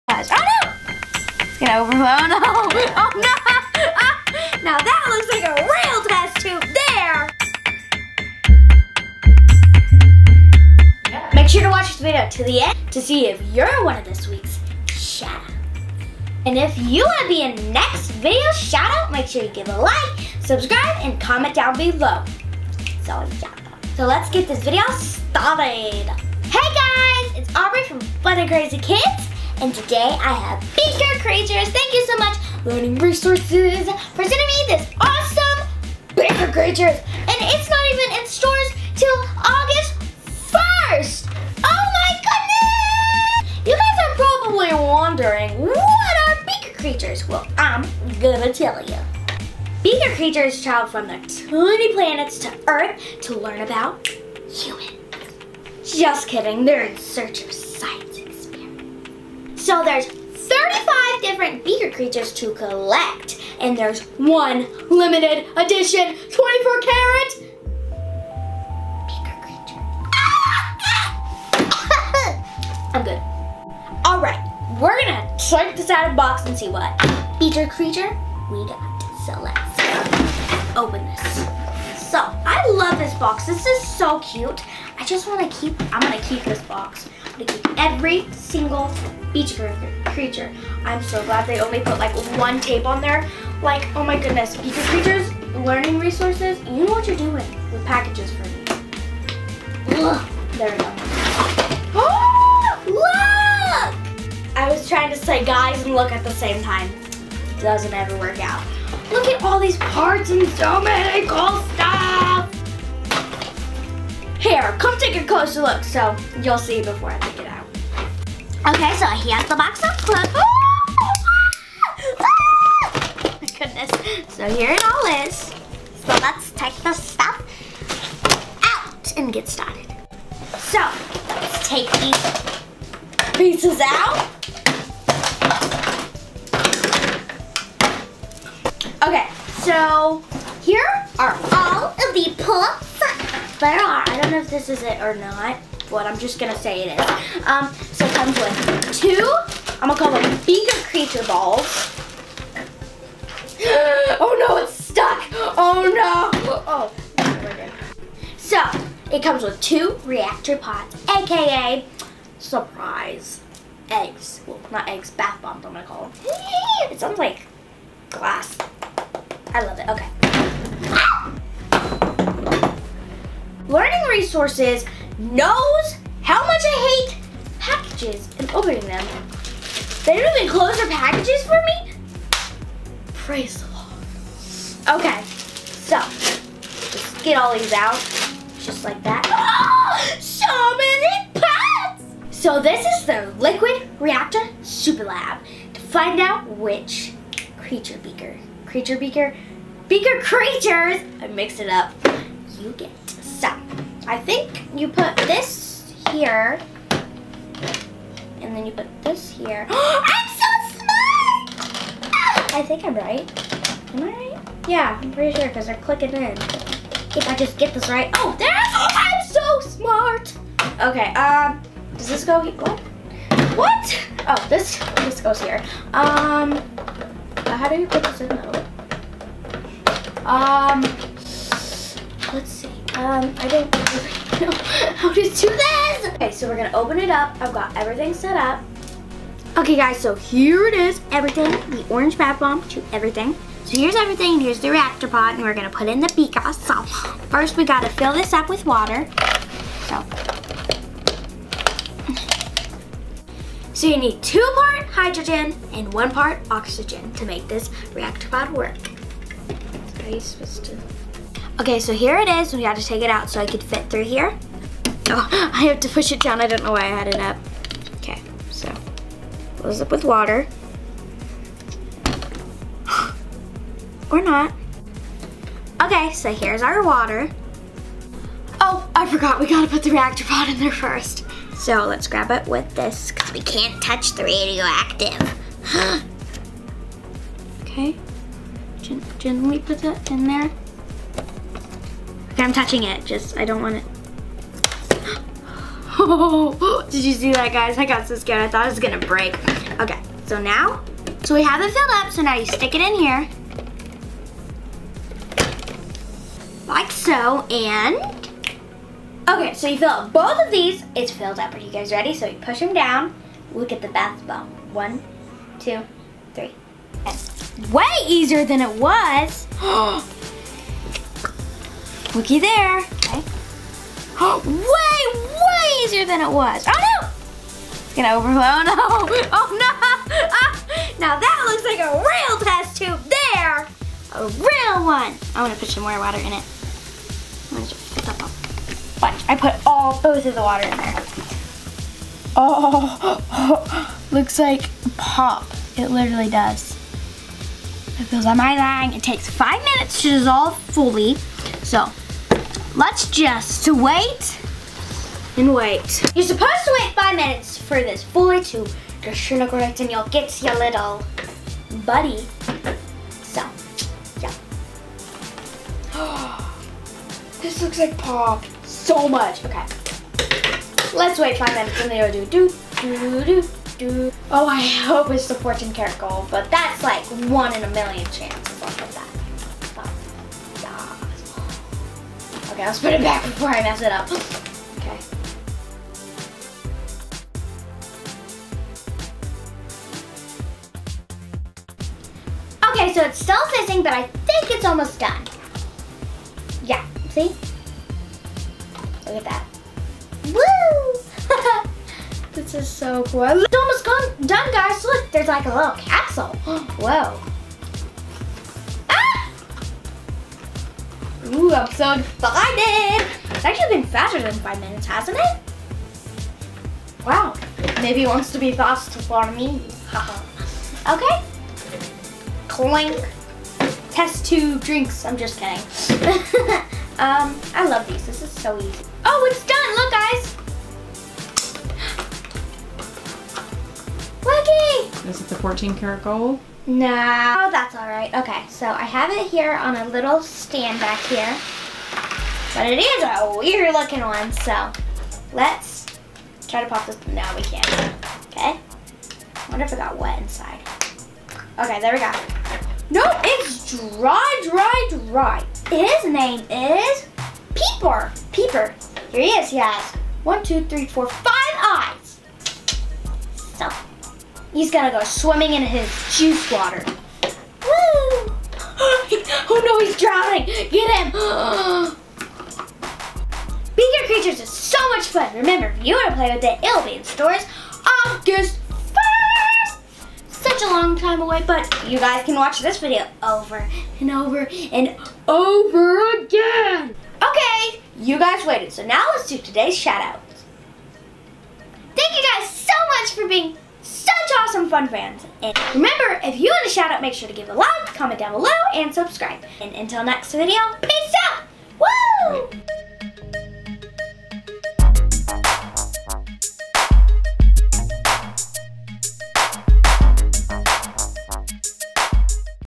Beaker Creatures SURPRISE TOYS FOR KIDS Fun Science Experiment!
Today Aubrey Opens Beaker Creatures  SURPRISE TOYS FOR KIDS Fun Science Experiment! Hi Friends! This new toy from learning resources is SO MUCH FUN! You get to do fun science experiments at home with the reactor lab and surprise eggs that have a hidden toy inside.

We love to read your letters! Send them to:
Fun and Crazy Kids